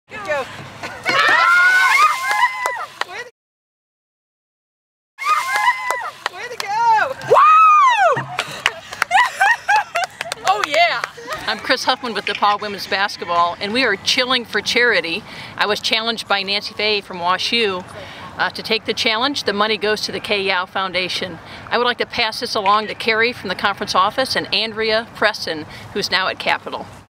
Chillin 4 Charity
DePauw women's basketball head coach Kris Huffman takes the Chillin 4 Charity Challenge at Blackstock Stadium on July 24, 2014. She was challenged by Washington University coach Nancy Fahey and, in turn, has challenged NCAC Executive Director Keri Alexander Luchowski and Capital assistant basketball coach Andrea Preston.